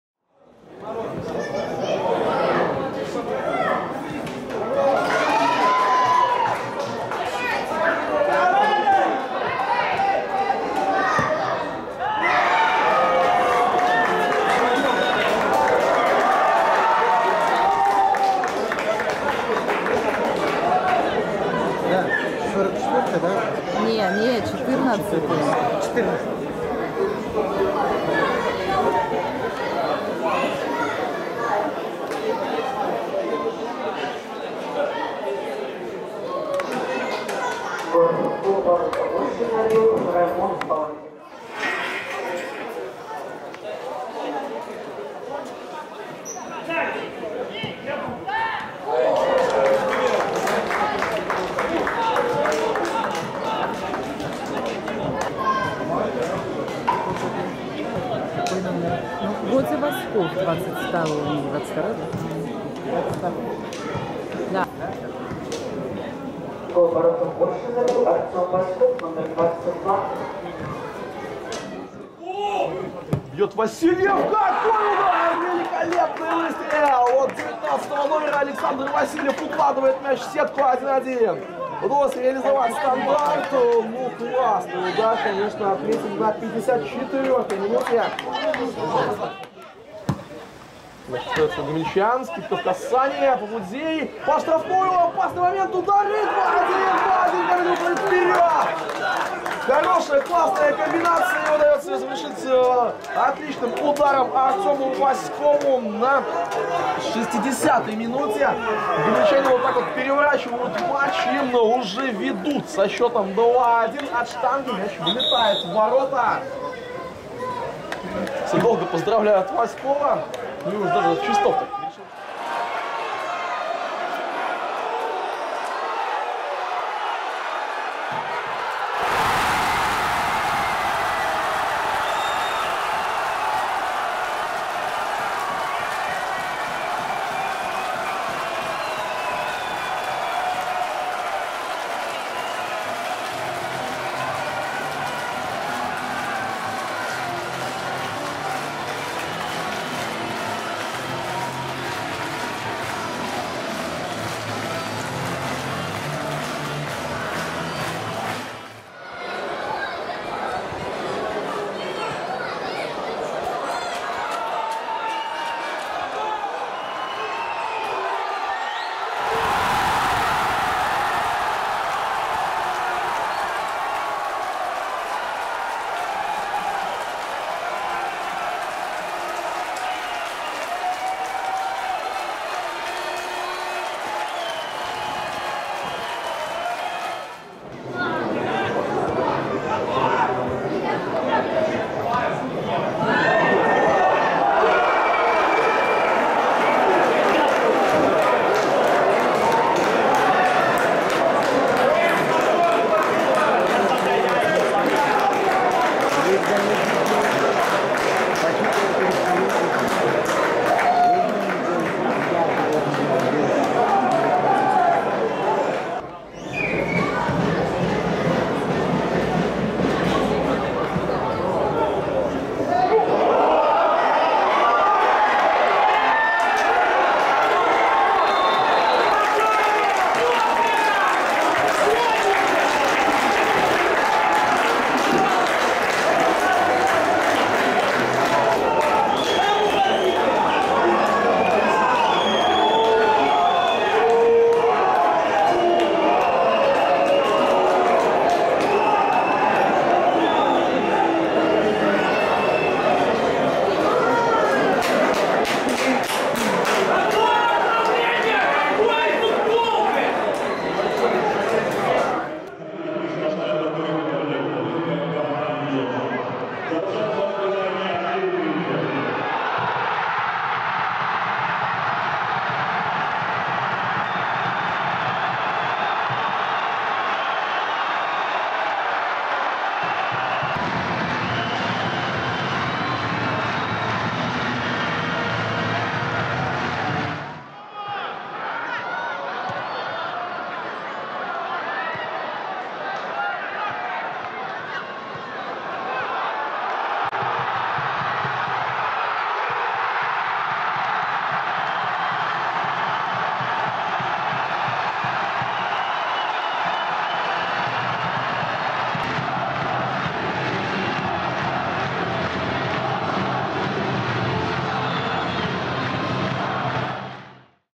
Голы "Гомеля" в сезоне-2021 | Часть 4
В четвертой части нашей голевой подборки вспомним игру с «Оршей», волевую победу над «Сморгонью», матч с «Шахтером», подаривший «зелено-белым» выход в 1/4 финала Кубка, а также два самых посещаемых матча чемпионата: с БАТЭ и минским «Динамо».